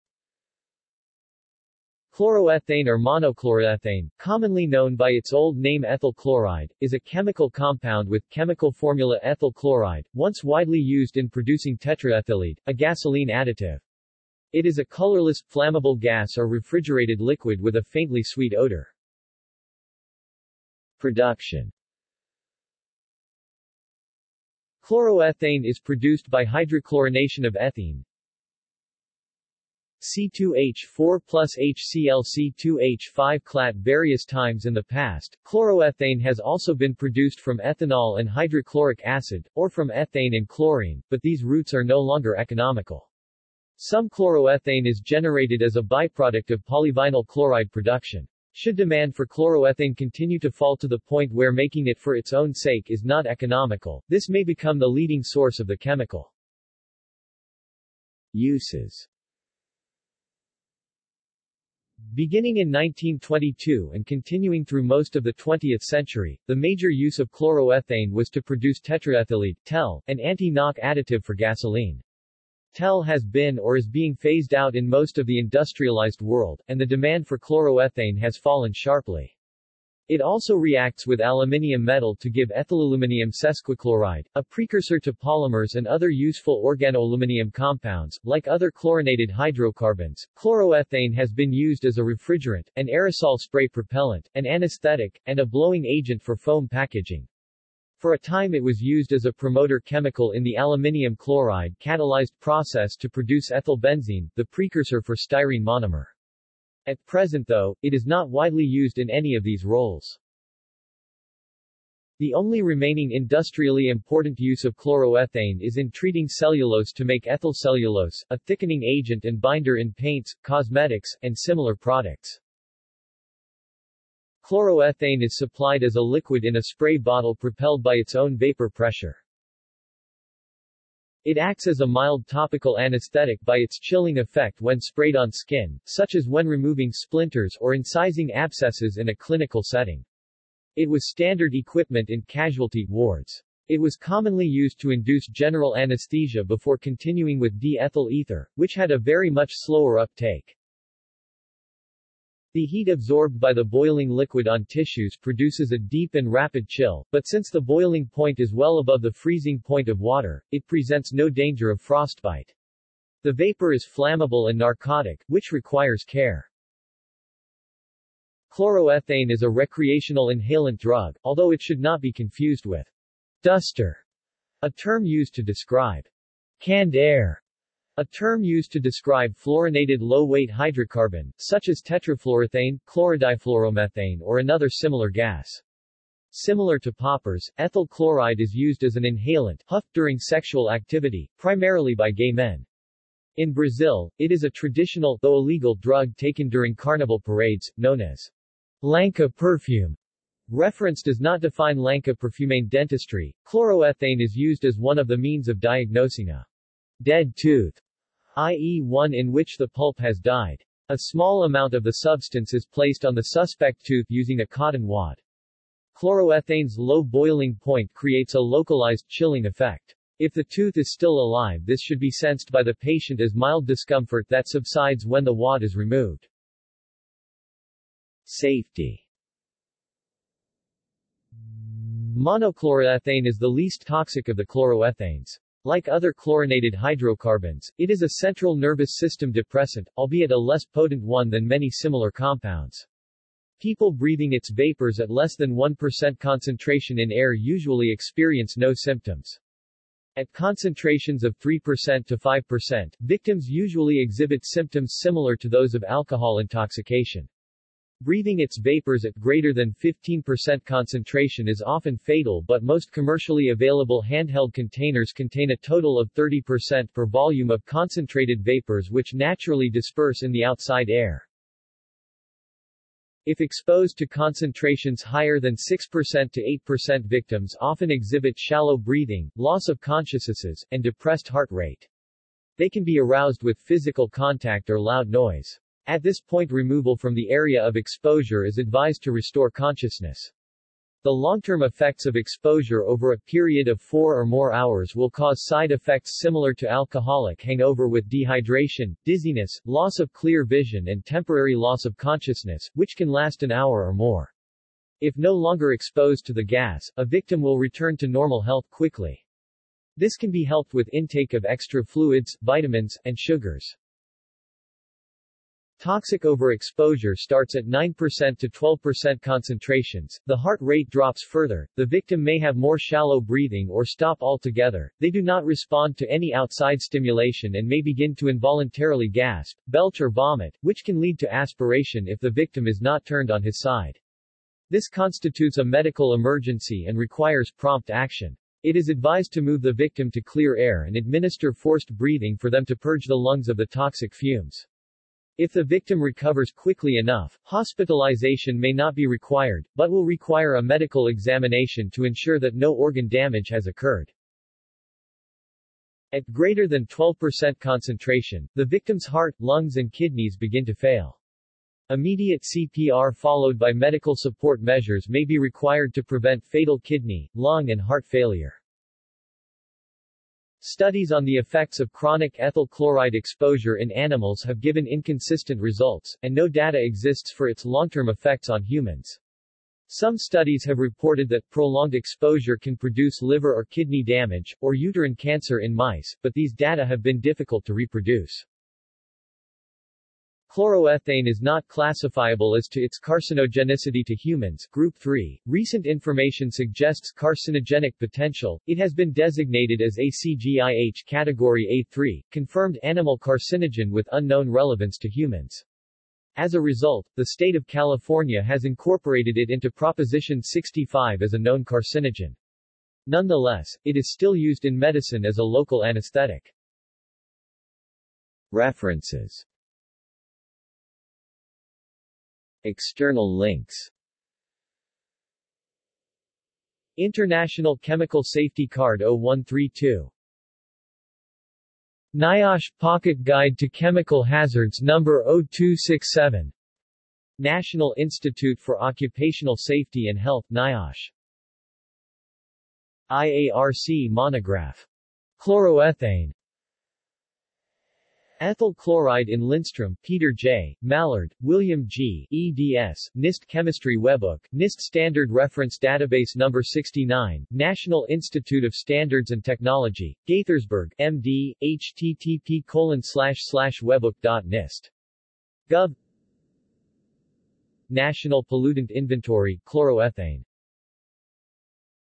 Chloroethane | Wikipedia audio article
This is an audio version of the Wikipedia Article:
Chloroethane

SUMMARY
Chloroethane or monochloroethane, commonly known by its old name ethyl chloride, is a chemical compound with chemical formula C2H5Cl, once widely used in producing tetraethyllead, a gasoline additive.  It is a colorless, flammable gas or refrigerated liquid with a faintly sweet odor.